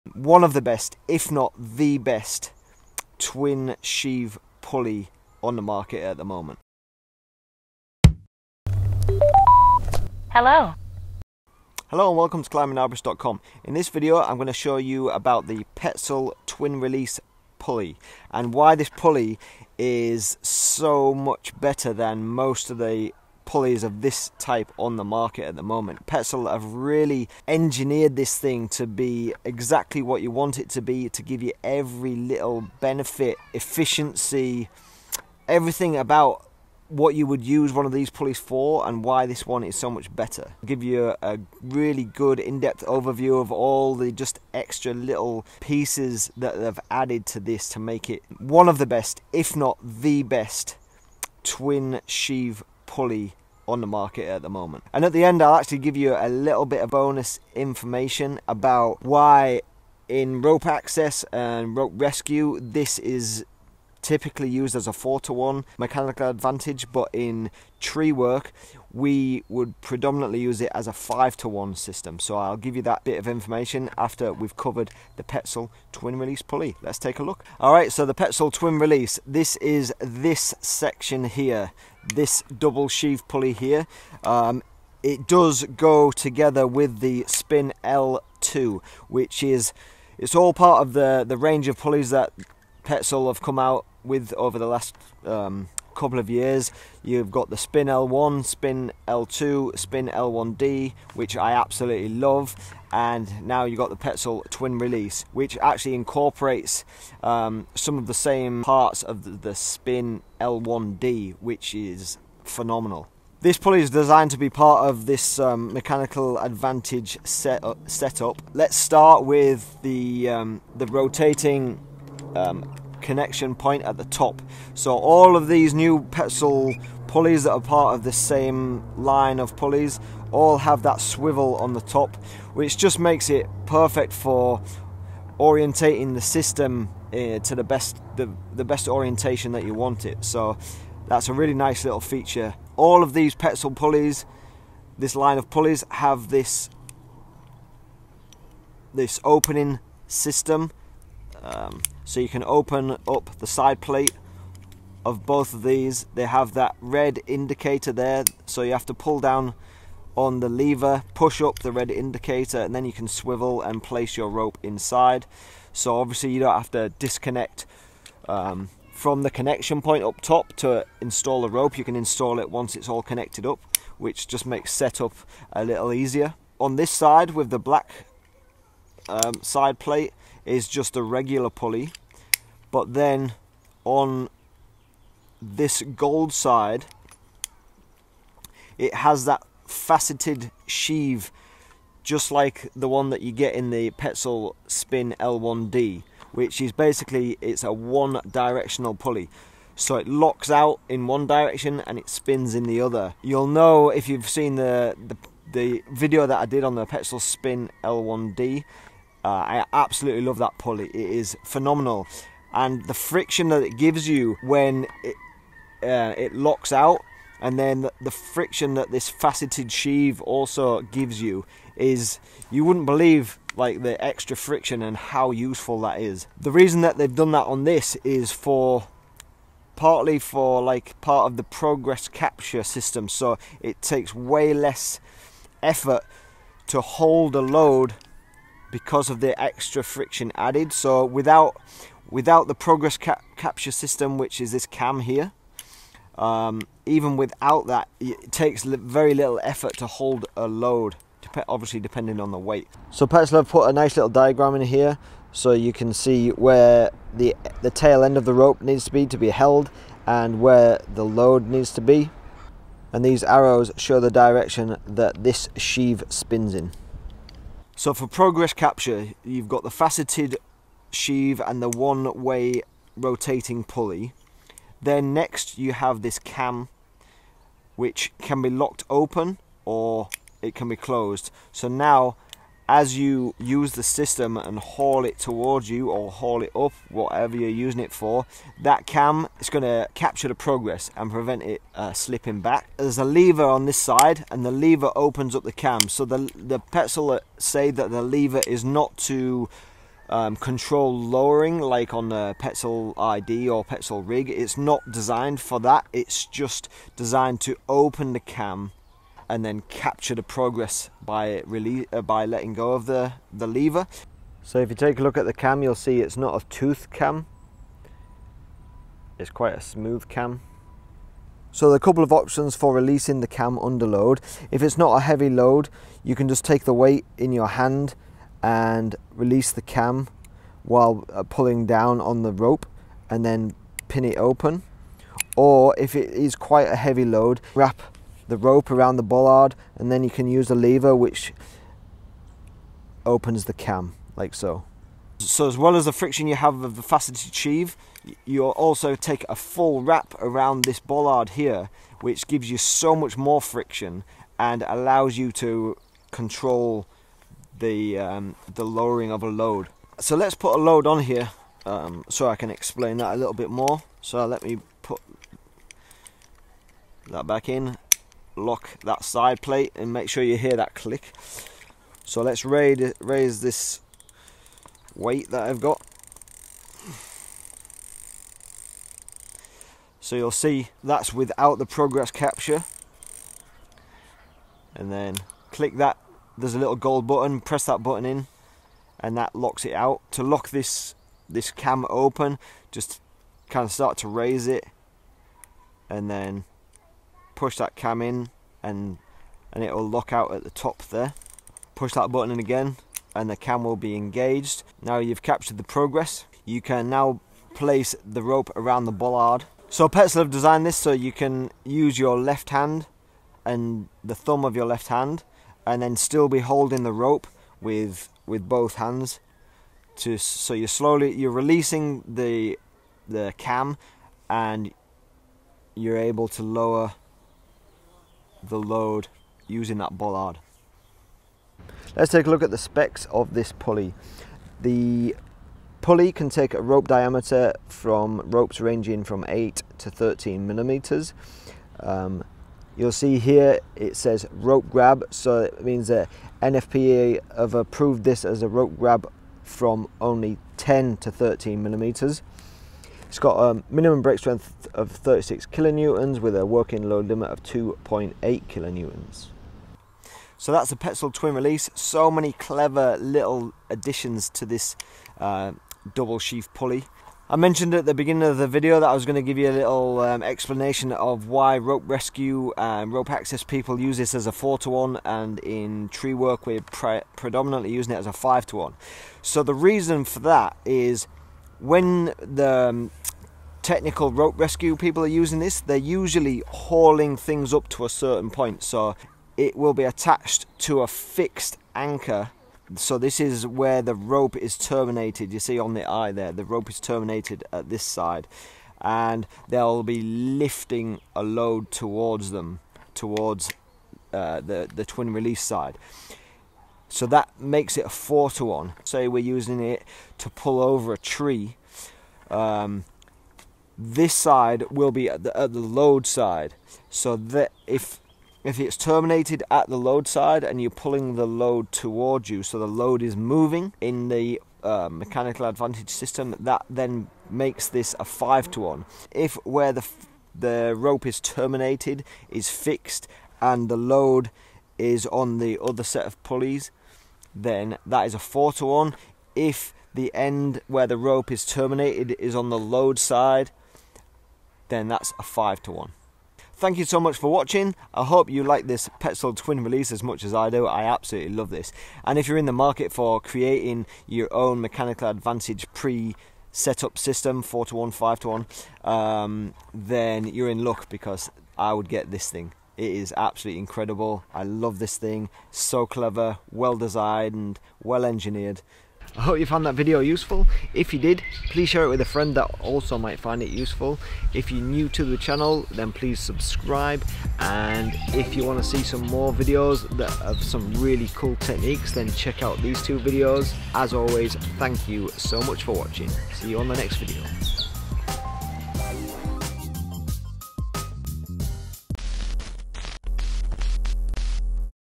REVIEW of the Petzl TWIN RELEASE pulley for mechanical advantage with built in progress capture
We review the 'TWIN RELEASE' from Petzl. This is a twin sheave pulley designed to be part of a mechanical advantage system, but it has some amazing extra features when compared with you typical twin sheave pulley.
We run through all the features of this product and how to use it.
We also give you some great bonus info at the end of the video about 4:1 & 5:1 Mechanical advantages.